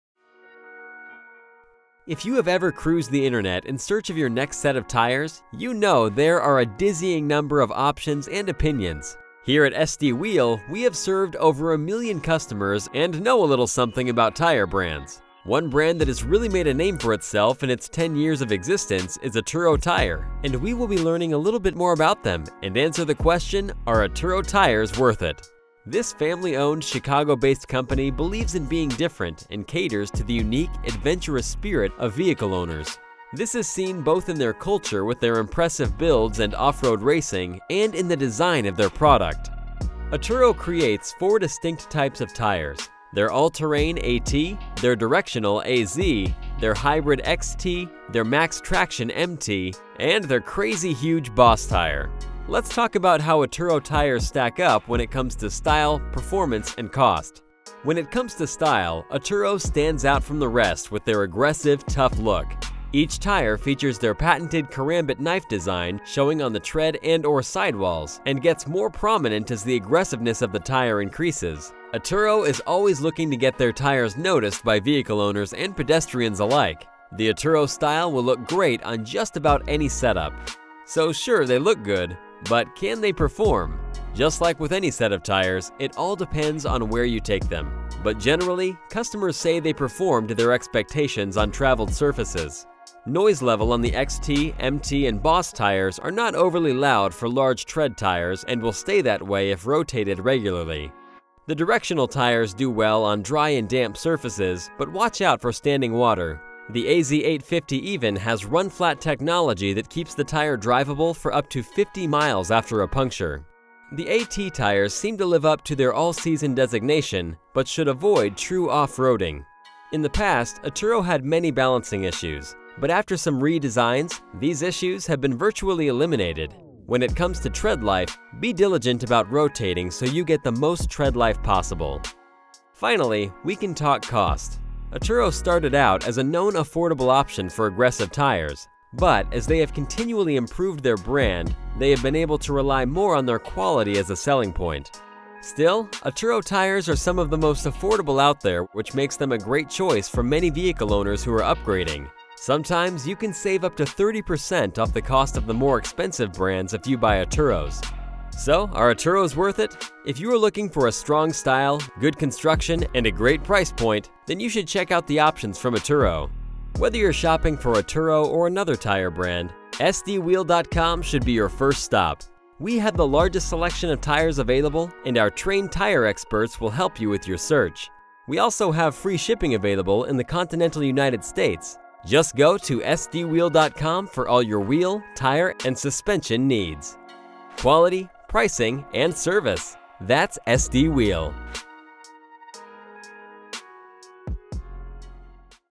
Are Atturo Tires Worth It?
Atturo Tires is a Chicago based company that has been around since 2009. They are one of the leaders of the tire industry.